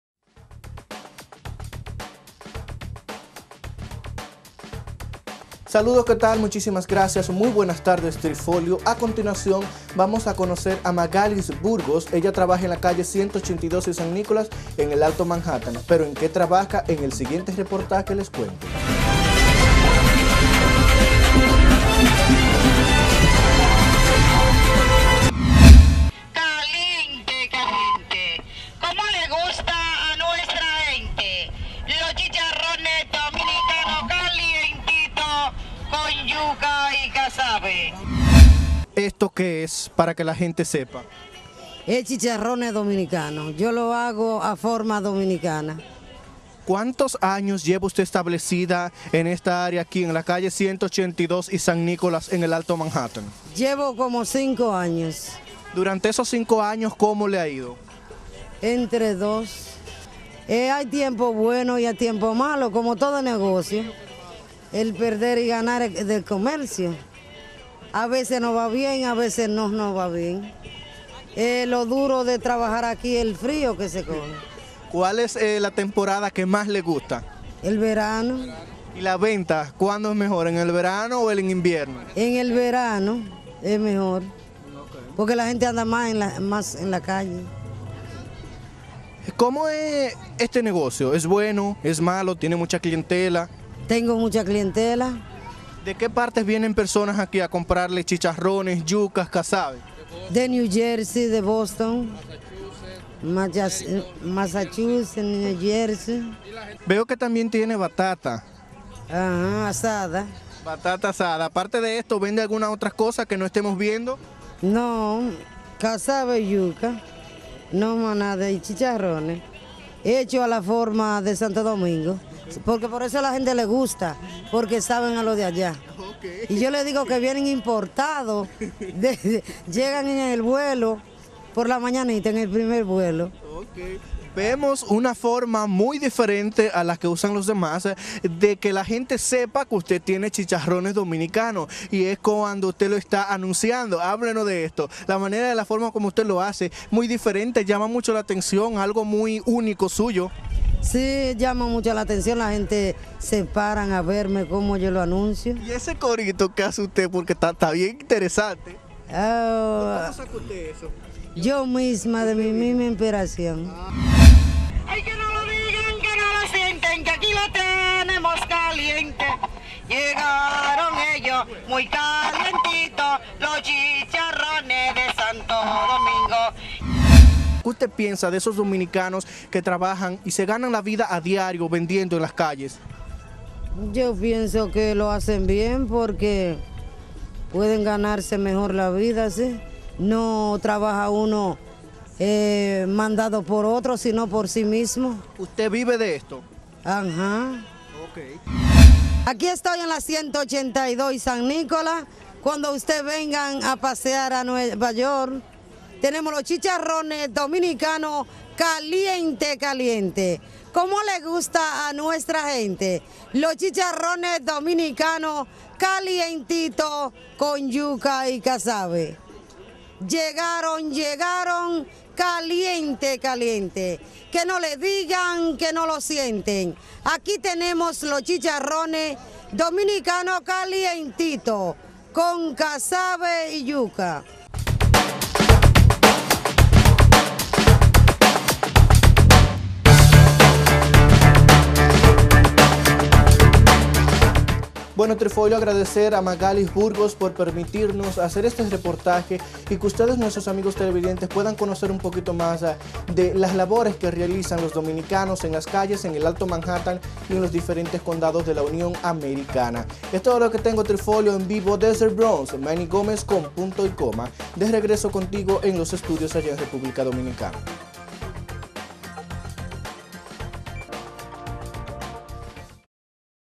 EL REPORTE DE MANNY GOMEZ MAGALY BURGOS Y SUS CHICHARRONES EN NY
Doña Magaly Burgos, dominicana residente en New York, explica para Con Punto & Coma..., cómo se gana la vida vendiendo Chicharrones en una calle del Alto Manhattan. En exclusiva para Con Punto & Coma... una producción audiovisual de Juan Cruz Triffolio. Un reporte especial de Manny Gómez.